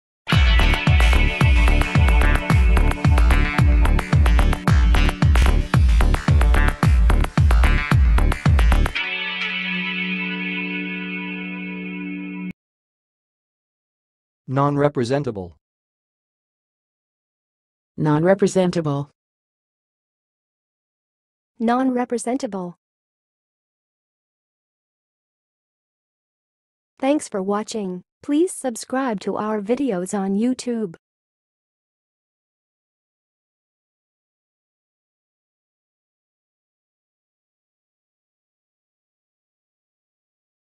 NONREPRESENTABLE - HOW TO SAY NONREPRESENTABLE?
NONREPRESENTABLE - How do you really pronounce Nonrepresentable? Find out here in 2023

Nonrepresentable is an adjective that describes something that cannot be accurately or adequately depicted or portrayed.
It means that there is no suitable or appropriate way to depict or represent a certain concept, idea, or phenomenon.
It can be used in various contexts, including art, mathematics, philosophy, and communication.

Pronunciation:
To pronounce nonrepresentable, you can say non-rep-ri-zen-tuh-buhl.
Other variants include nahn-rep-ri-zen-tuh-buhl and non-ri-pri-zen-tuh-buhl.

Test yourself by speaking the following examples:
1. The artist experimented with nonrepresentable art that challenged traditional forms of representation
2. In mathematics, there are certain numbers that are nonrepresentable in certain numeral systems
3. Some philosophers argue that consciousness is nonrepresentable through language
4. The concept of infinity is often seen as nonrepresentable due to its abstract nature
5. The nonrepresentable emotions that she experienced after the accident were difficult for her to articulate
6. The complexity of certain phenomena makes them nonrepresentable in simple models or diagrams